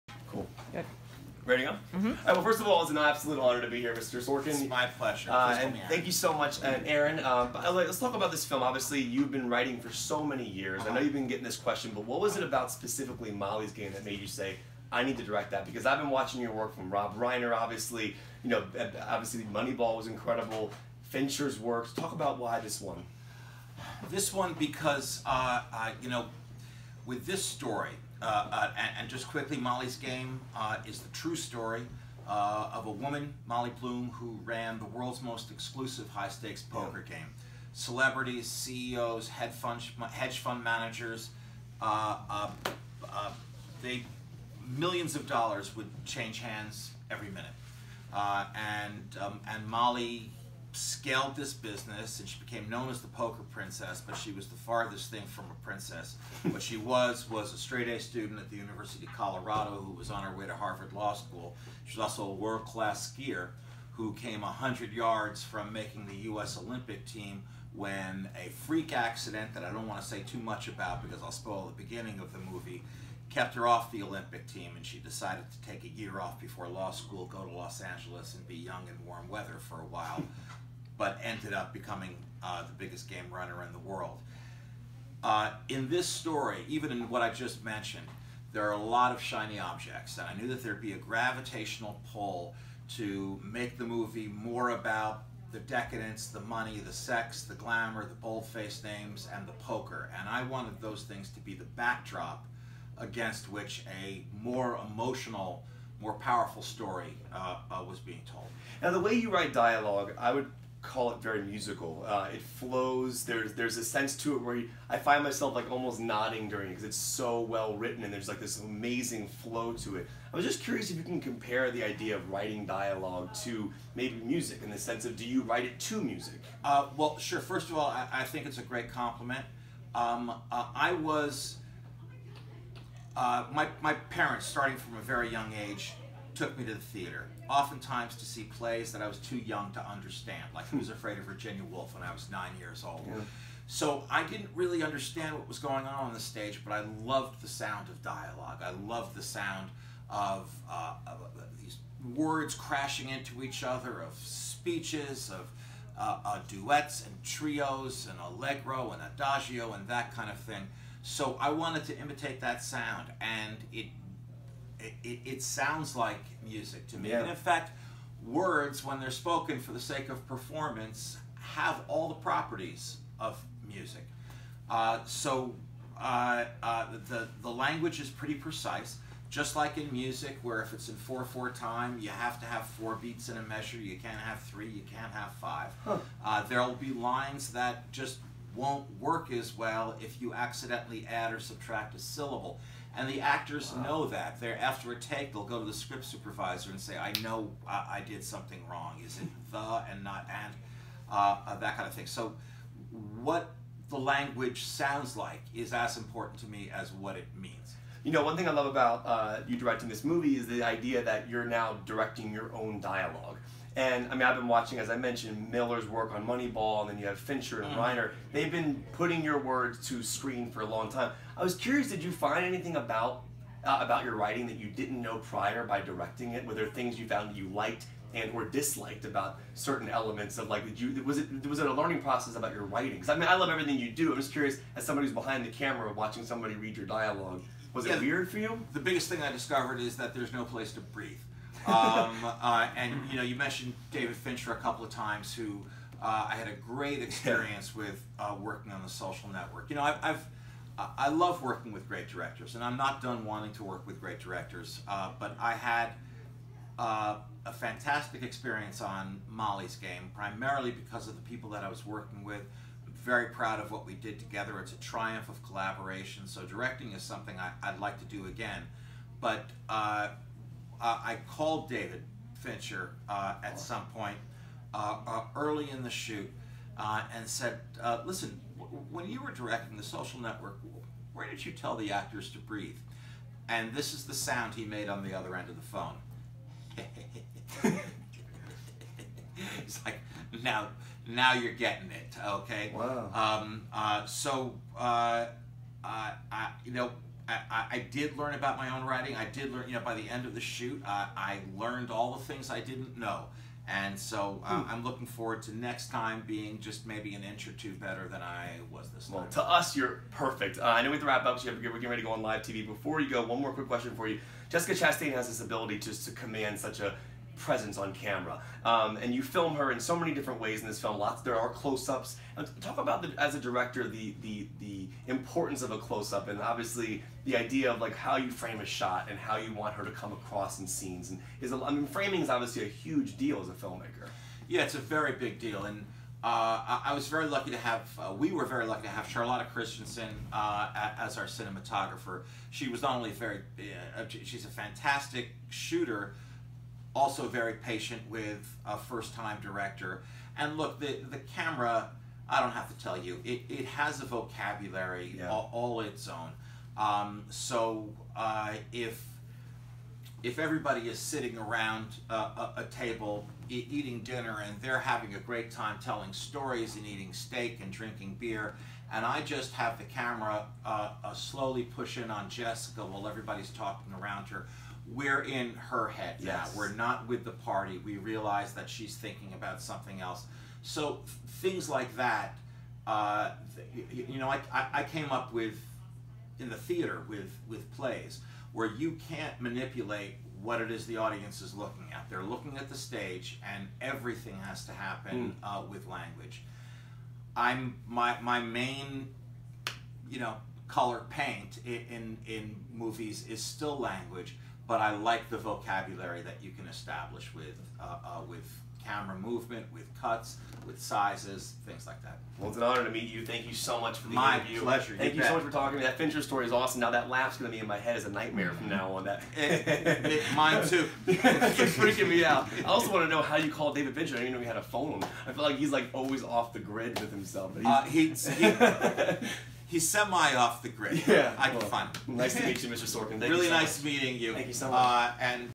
Aaron Sorkin interview for MOLLY'S GAME - The Social Network, David Fincher
Fox 5's entertainment reporter, Kevin McCarthy (@KevinMcCarthyTV), sat down with Aaron Sorkin to talk about his directorial debut, "Molly's Game."